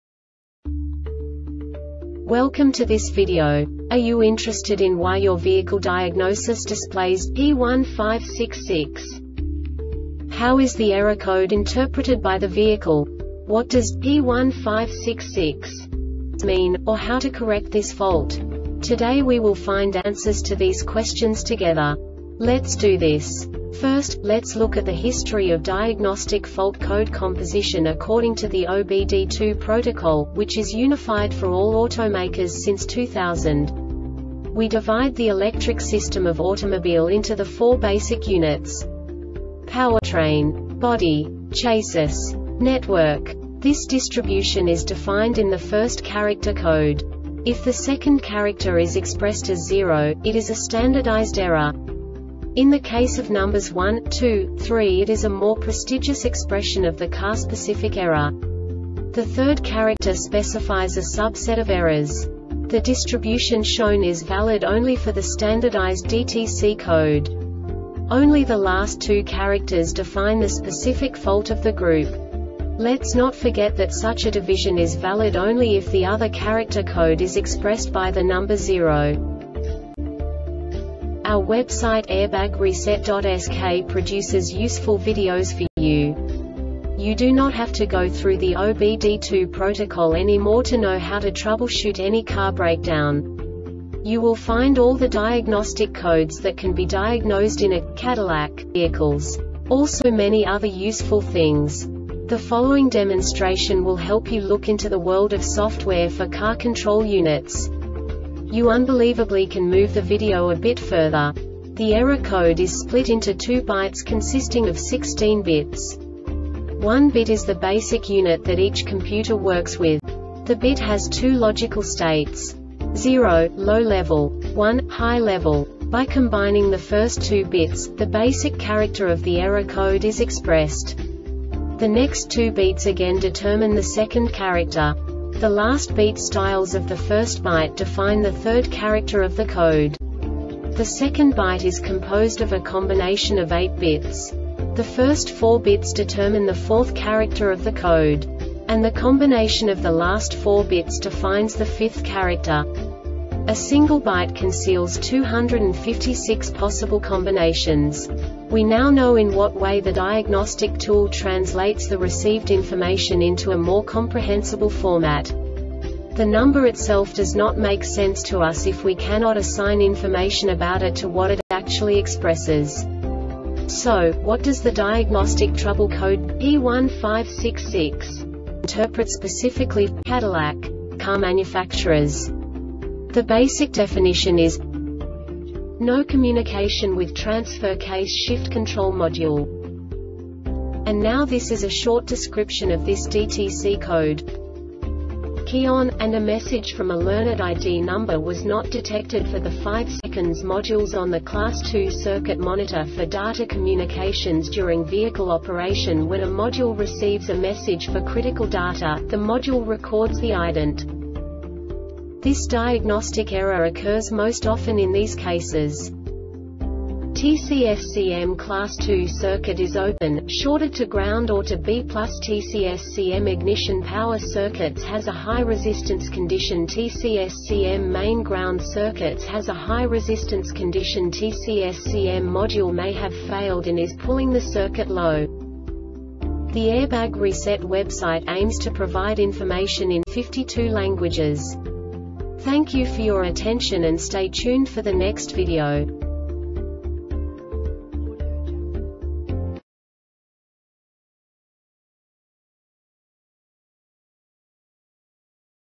DTC cadillac P1566 Short Explanation
Contents:
0:21 Basic DTC analysis according to OBD2 protocol standard.
1:48 Insight into programming
2:58 A diagnostically understandable description of the Diagnostic Trouble Code
3:05 Basic error definition

Make:
Cadillac
Code:
P1566
Definition:
No Communication With Transfer Case Shift Control Module
Description:
Cause:
TCSCM Class 2 circuit is open,shorted to ground or to B+TCSCM ignition power circuit(s) has a high resistance conditionTCSCM main ground circuit(s) has a high resistance conditionTCSCM (module) may have failed and is pulling the circuit low